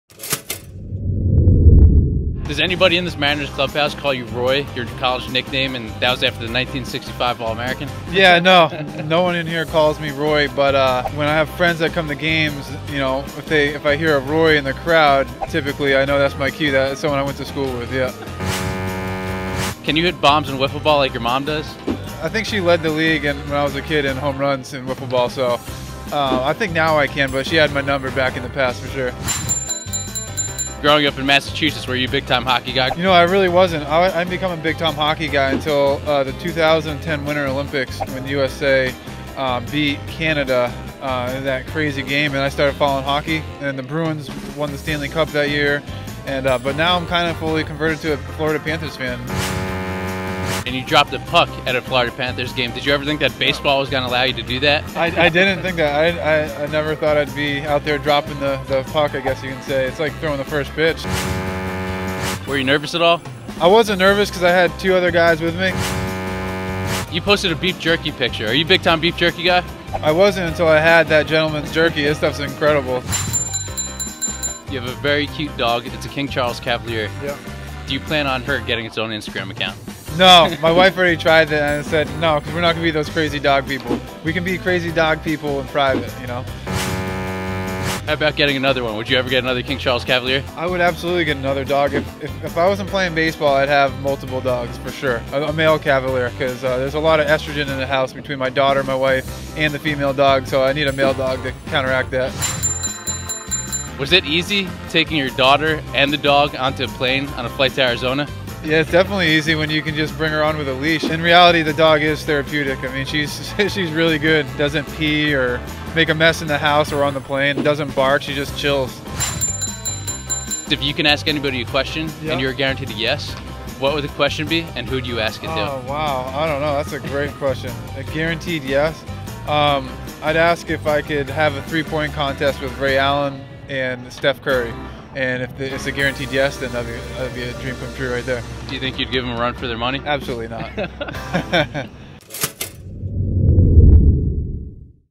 Steve Chisek's love for dogs, NHL fandom, wiffle ball skills & more - YES or No
Get to MLB pitcher Steve Chisek as he talks about his love for dogs, liking for the NHL, wiffle ball skills and more with YES Network's Matthew Stucko.